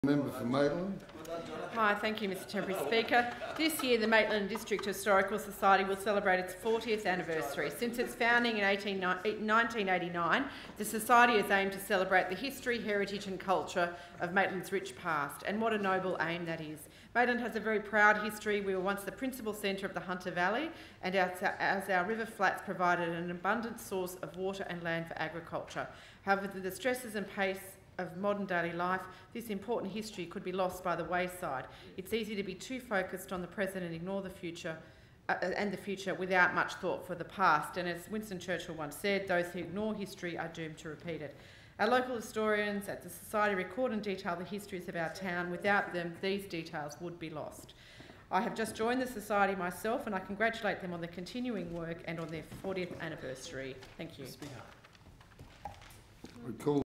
Maitland and District Historical Society Fortieth Anniversary I CRS I 1 June 2017
Ms JENNY AITCHISON  ( Maitland ) ( 16:24  ): This year the Maitland and District Historical Society will celebrate its fortieth anniversary. Since its founding in 1989, the society has aimed to celebrate the "history, heritage and culture of Maitland's rich past." And what a noble aim that is. Maitland has a proud history. We were once the principal centre of the Hunter Valley as our river flats provided an abundant source of water and land for agriculture. However, with the stresses and pace of modern-day life, this important history could fall by the wayside and be lost. It is too easy to be focused on the present and the future without giving much thought to the past. As Winston Churchill once said, "Those who ignore history are doomed to repeat it." Our local historians at the society record and detail the histories of our town. Without them, these details would be lost. I have just joined the society and I congratulate the society on its continuing work and its fortieth anniversary.